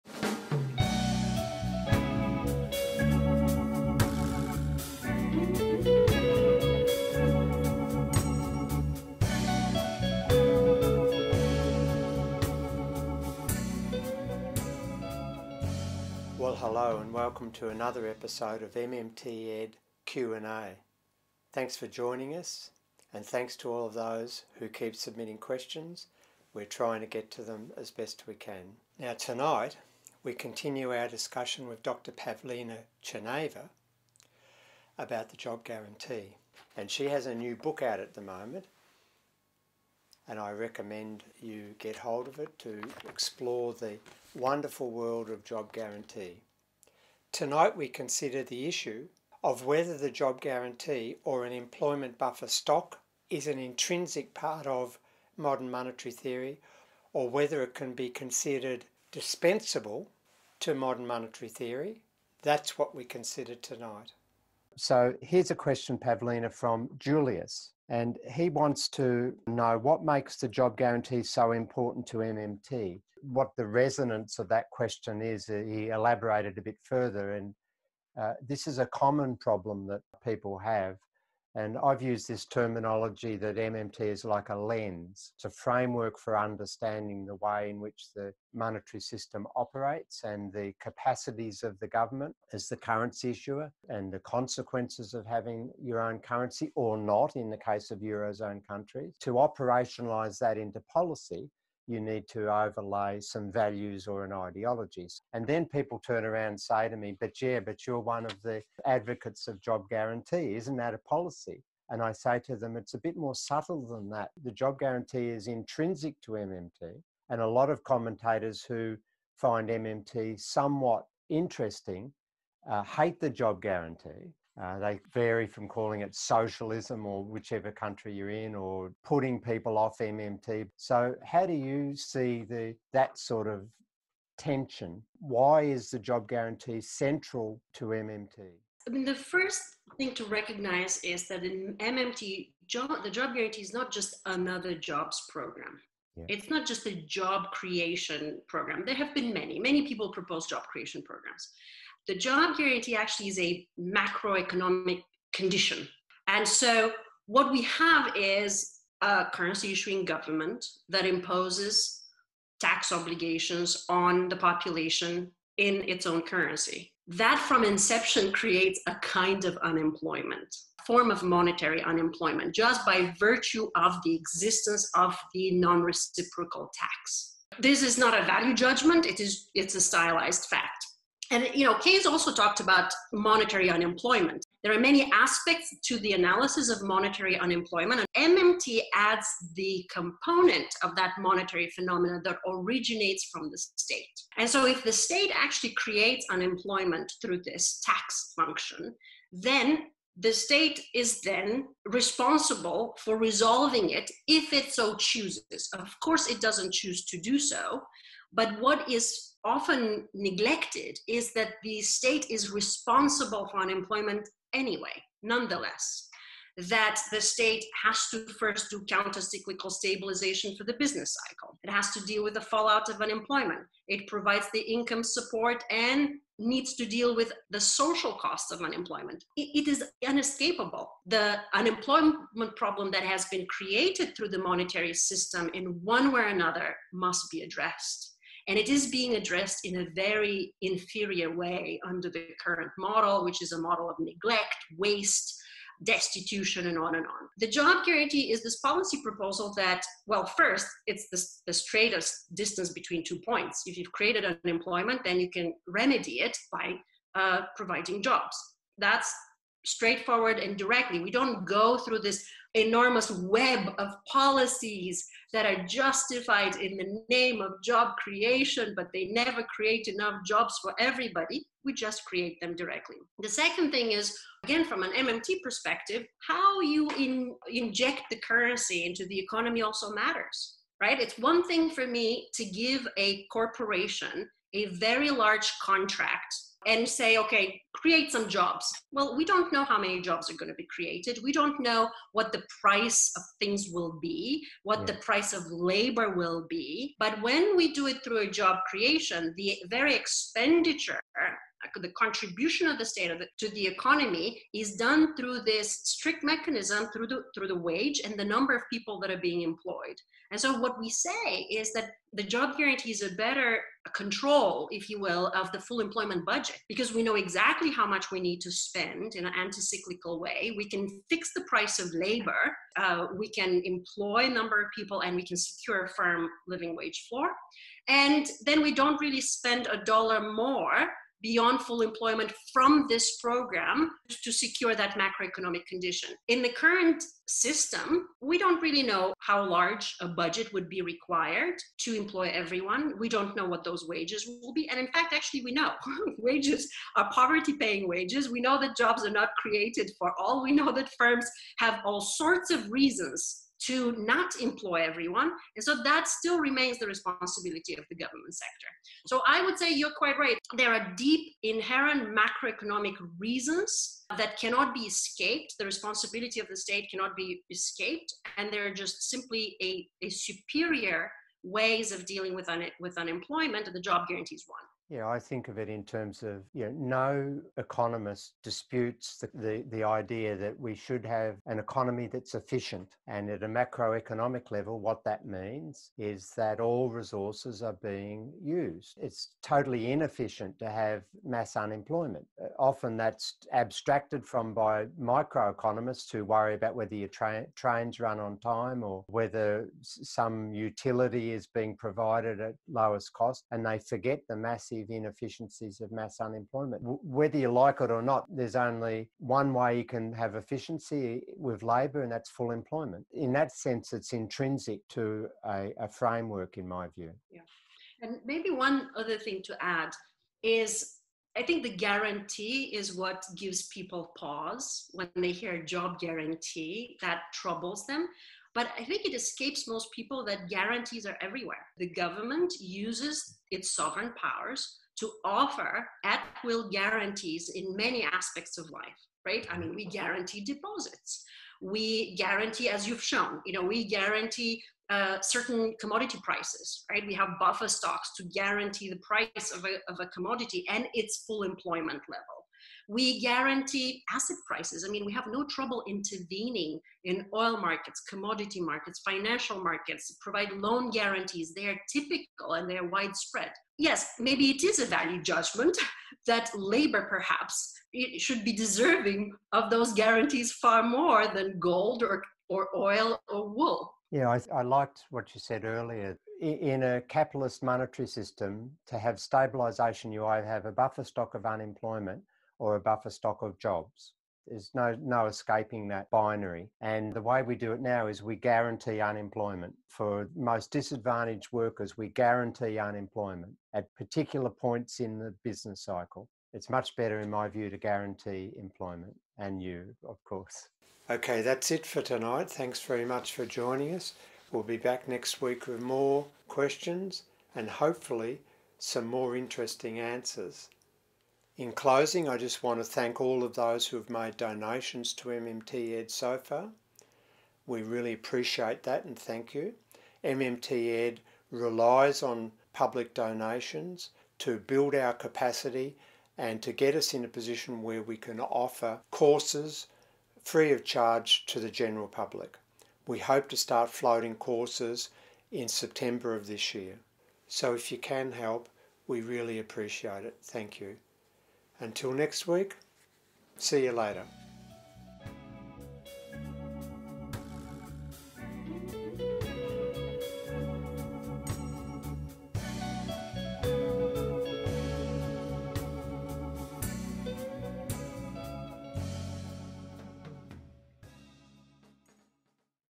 MMTed Q&A Episode 6
This is Episode 6 in the first season of MMTed Q&A where MMT experts answer questions submitted by the public.

The host is Professor William Mitchell.

This week's guest: Dr Pavlina Tcherneva, Bard College, US and author of 'The Case for the Job Guarantee'.